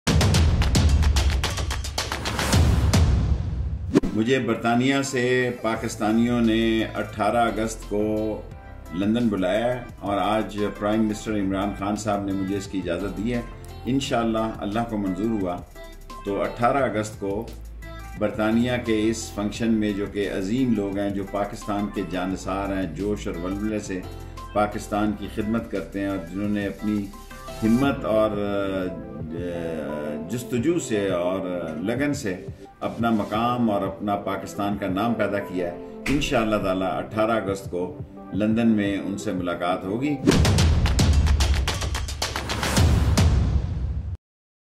Federal Minister Railway Sheikh Rasheed Ahmed coming to 10th Pakistan Achievement Awards 2019
Federal Minister Railway Sheikh Rasheed Ahmed coming to 10th Pakistan Achievement Awards 2019 Sunday 18th August in London